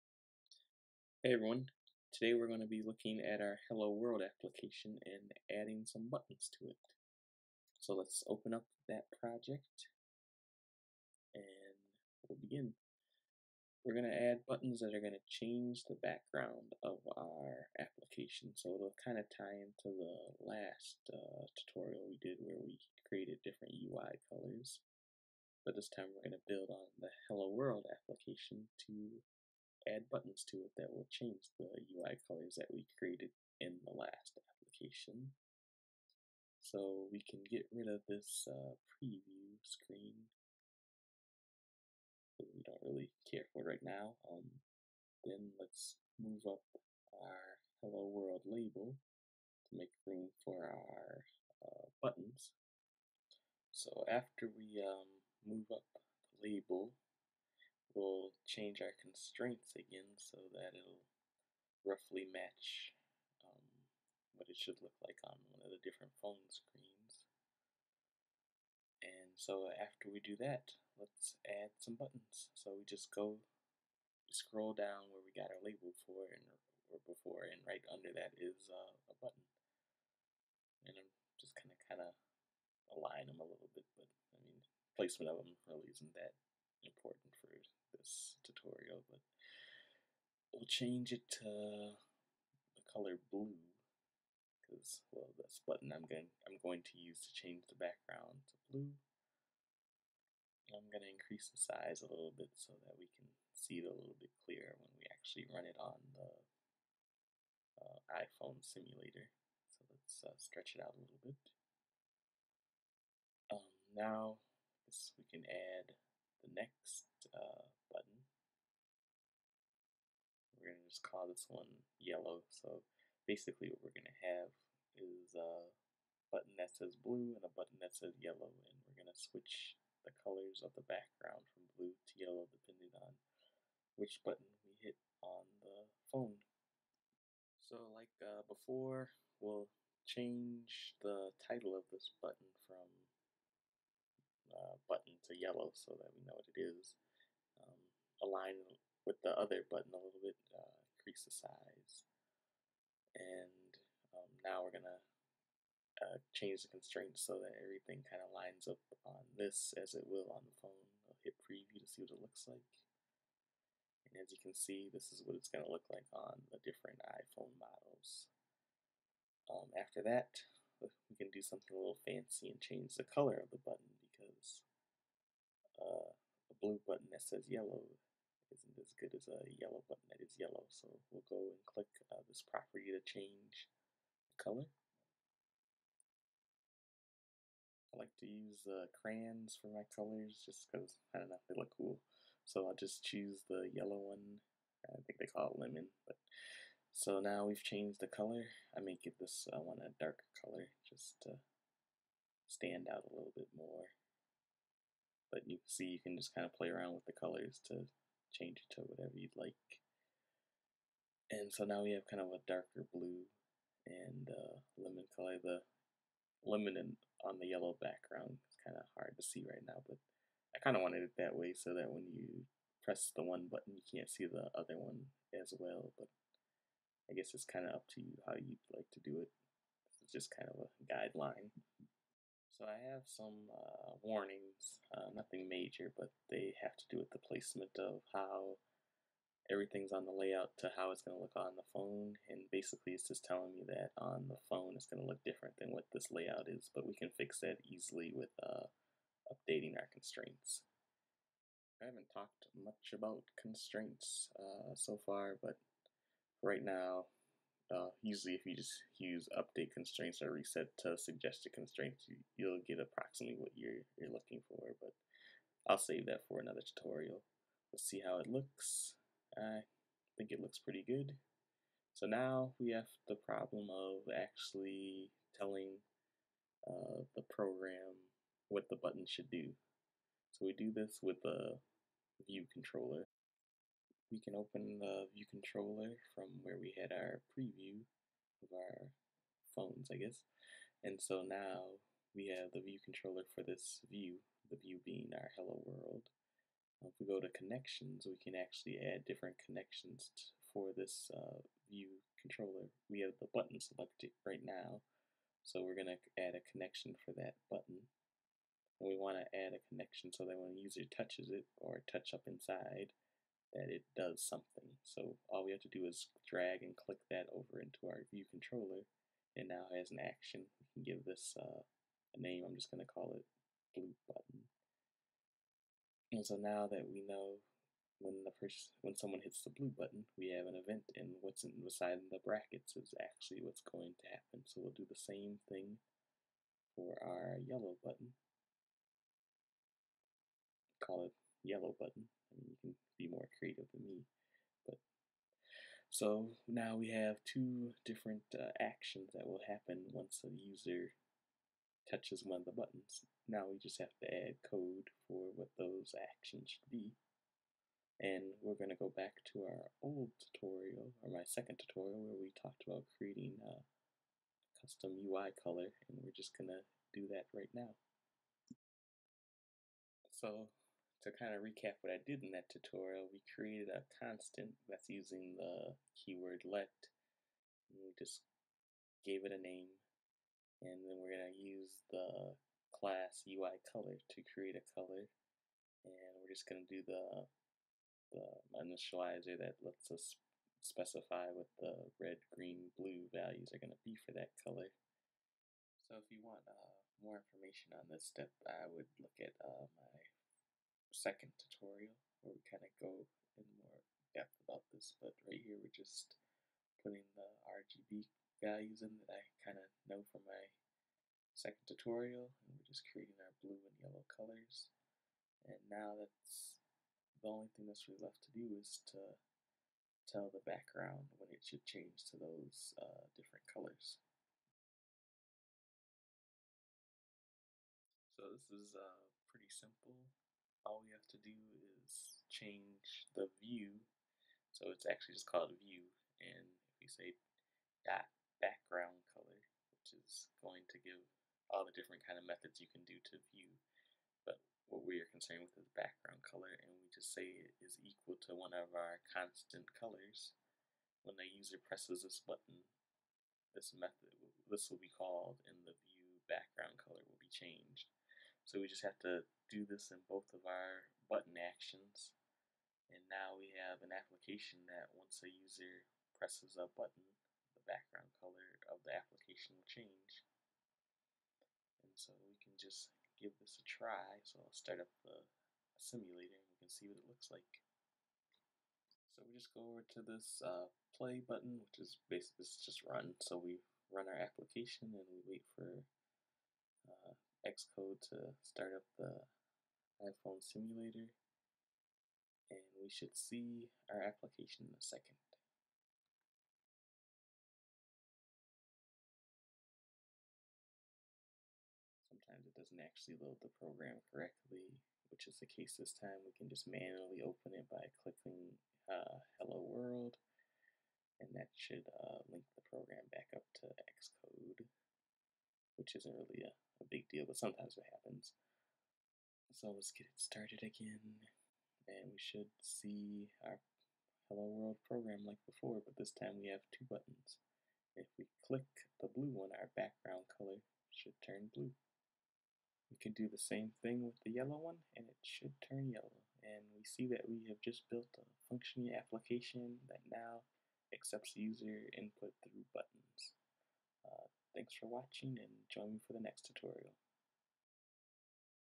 iOS Tutorial 3: Button Fun (Swift + Xcode) [LOW VOLUME]
*LOW VOLUME* - Will release update
This tutorial will go over adding Buttons to the Hello World application.
We will change the background color depending on the button pressed.
This lesson will build on tutorial 1 & 2